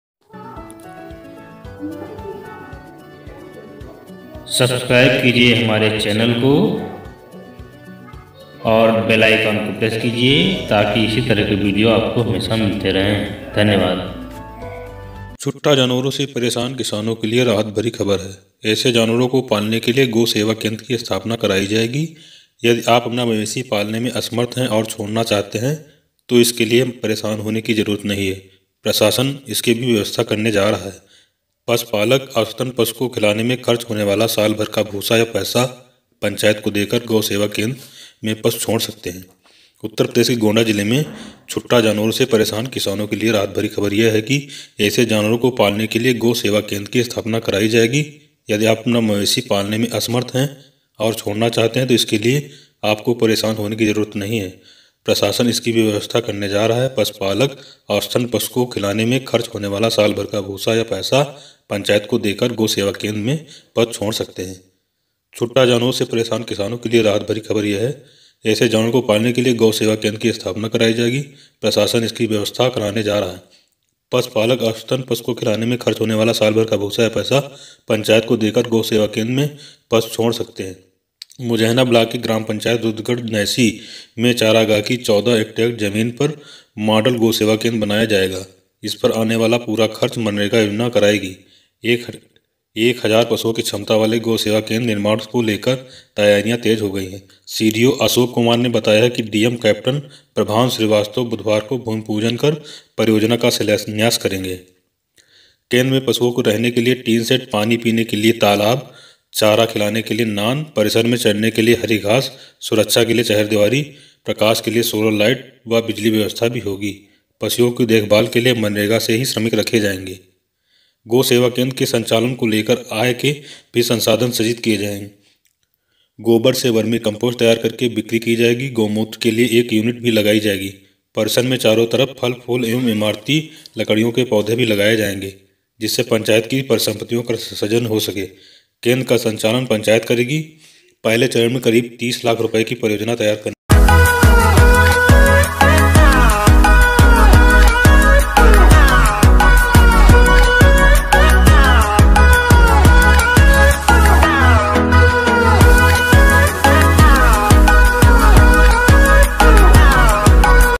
Mnrega, yujana, Se,Kishano, Ko rahat।आवारा पशुओं, से छुटकारा। मजदूरों को भी होगा लाभ।।
Mnrega PMAYG Ujjwala ROJGARSEWAKछुट्टा जानवरों से परेशान किसानों के लिए राहत भरी खबर है। ऐसे जानवरों को पालने के लिए गो-सेवा केंद्र की स्थापना कराई जाएगी। यदि आप अपना मवेशी पालने असमर्थ हैं और छोड़ना चाहते हैं तो इसके लिए आपको परेशान होने की जरूरत नहीं है। प्रशासन इसकी भी व्यवस्था करने जा रहा है। पशुपालक औसतन पशु को खिलाने में खर्च होने वाला साल भर का भूसा या फिर पैसा पंचायत को देकर गो-सेवा केंद्र में पशु छोड़ सकते हैं।...

गोंडा : छुट्टा जानवरों से परेशान किसानों के लिए राहत भरी खबर है। ऐसे जानवरों को पालने के लिए गो-सेवा केंद्र की स्थापना कराई जाएगी। प्रशासन इसकी भी व्यवस्था करने जा रहा है। पशुपालक औसतन पशु को खिलाने में खर्च होने वाला साल भर का भूसा या फिर पैसा पंचायत को देकर गो-सेवा केंद्र में पशु छोड़ सकते हैं। मुजेहना ब्लॉक की ग्राम पंचायत रुद्रगढ़ नौसी में चारागाह की 14 हेक्टेयर जमीन पर मॉडल गो-सेवा केंद्र बनाया जाएगा। इसपर आने वाला पूरा खर्च मनरेगा योजना उठाएगी। एक हजार पशुओं की क्षमता वाले गो-सेवा केंद्र निर्माण को लेकर तैयारियां तेज हो गई हैं। सीडीओ अशोक कुमार ने बताया कि डीएम कैप्टन प्रभांशु श्रीवास्तव बुधवार को भूमि पूजन कर परियोजना का शिलान्यास करेंगे।